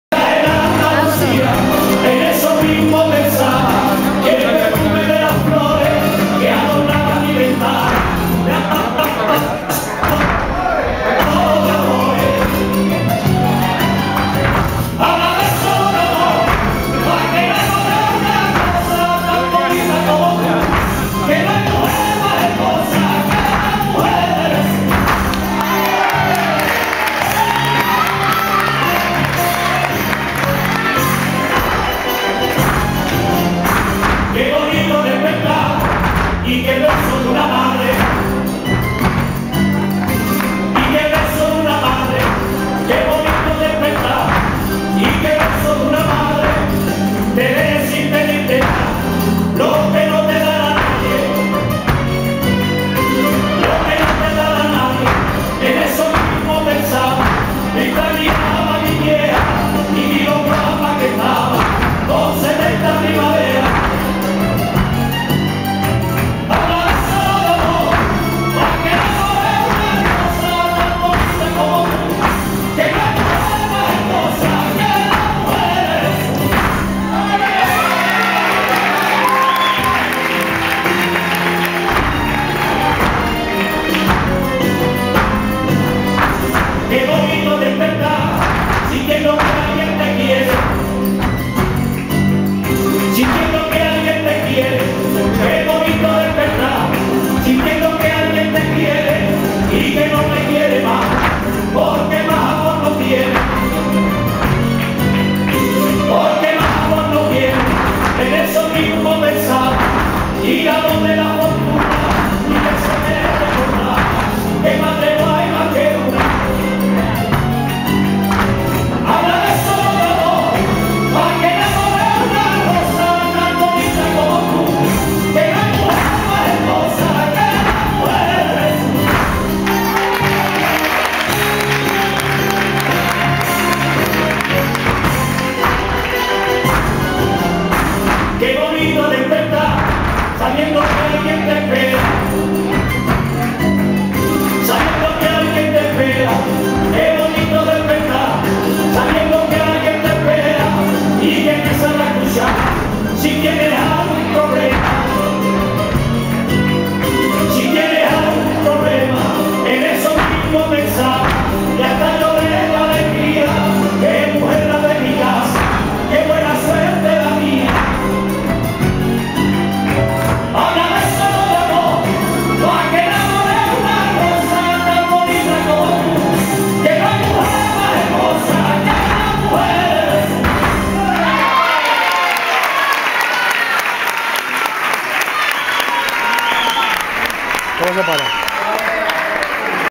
Baile de seis.MP4
Excelente por el Grupo de Baile de la Casa de Andalucía en Logroño!!!